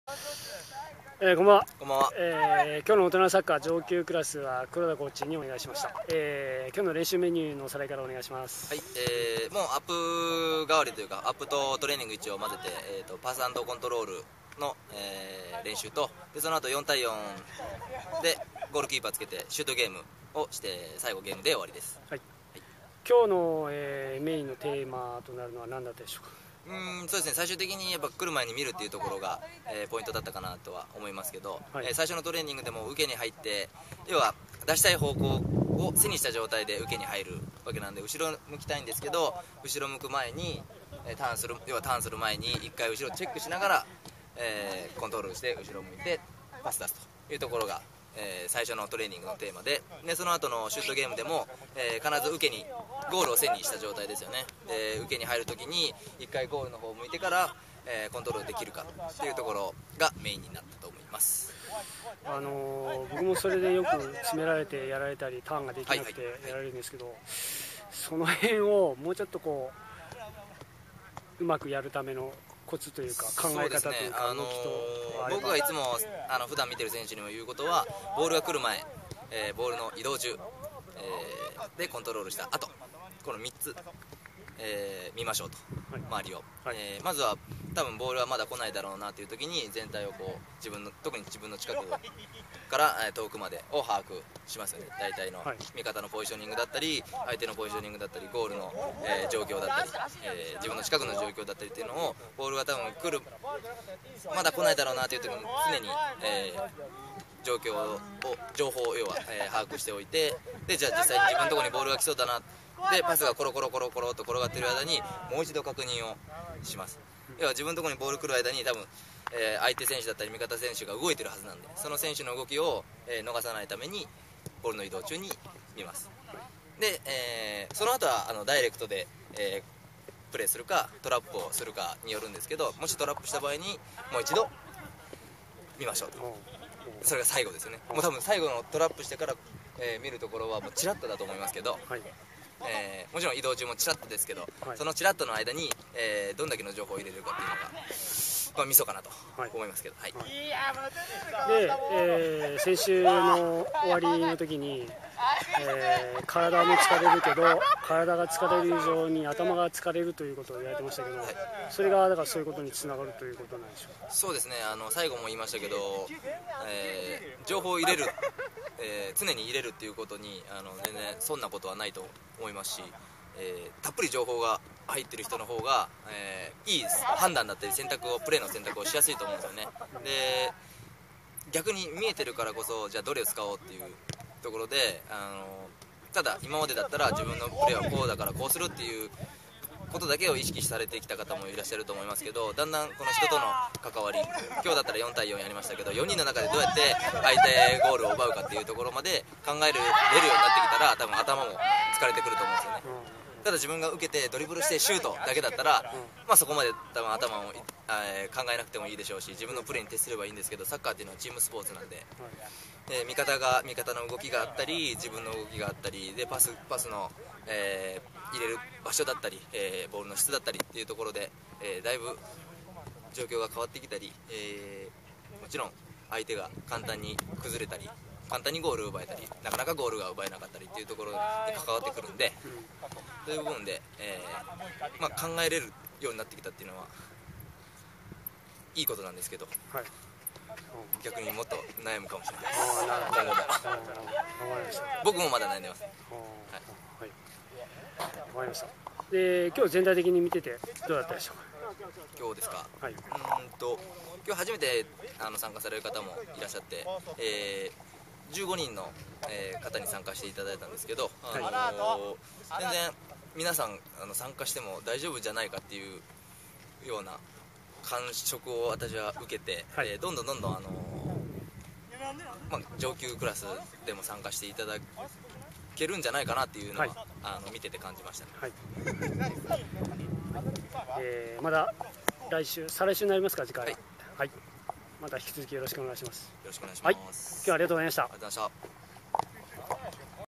2010/05/20大人のサッカー上級クラス 黒田コーチ！
今夜の「大分トリニータ」大人のサッカー、上級クラスは黒田コーチ。最近はプレー中にホイッスル吹いてプレーを止め、細かい指示を出してくれたりして「本気度」が上がってます、「体も疲れるがそれ以上にアタマが疲れるプレーを」が名言。今夏はその秘訣を教えてくれています。